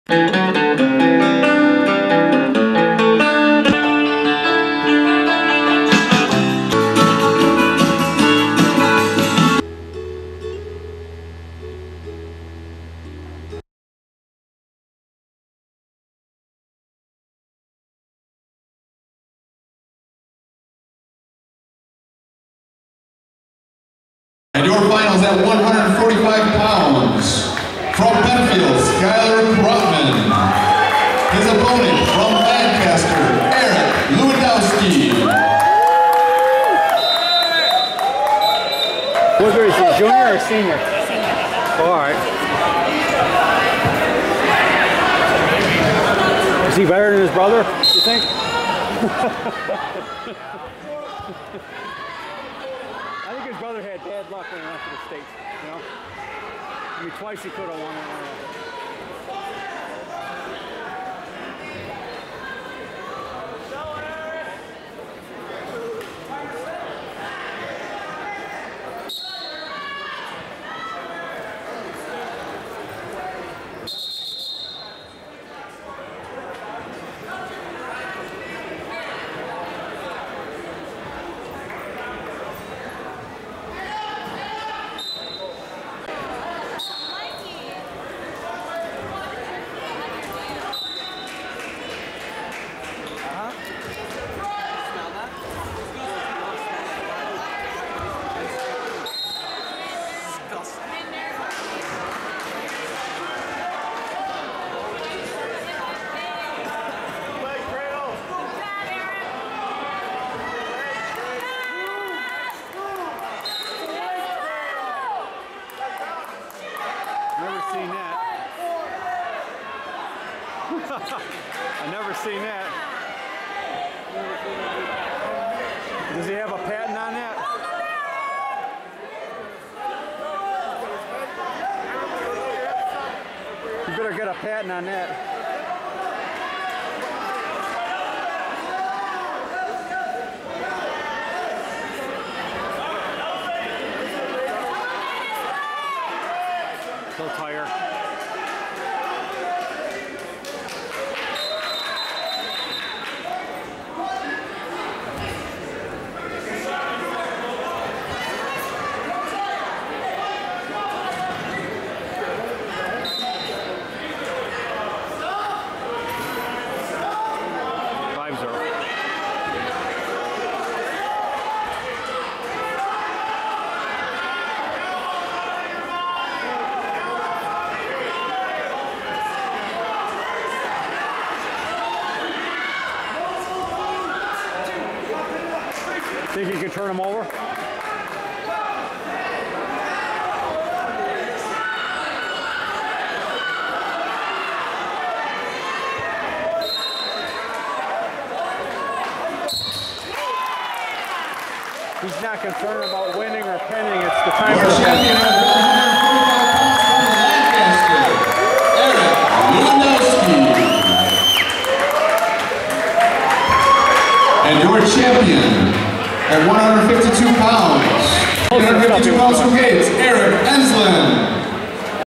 Whoifwhat: 2012 Penfield Hunter Wrestling 145Lbs.
@ 145 Lbs Final 2 good wrestlers : Eric Lewandowski (Lancaster)  Pin 1:58
Skyler Kropman (Penfield) in must see match ! Eric dominates with takedown & pinning combinations I never saw before ! In background 152 Lbs. match Eric Ensslin (Gates) beats Webster !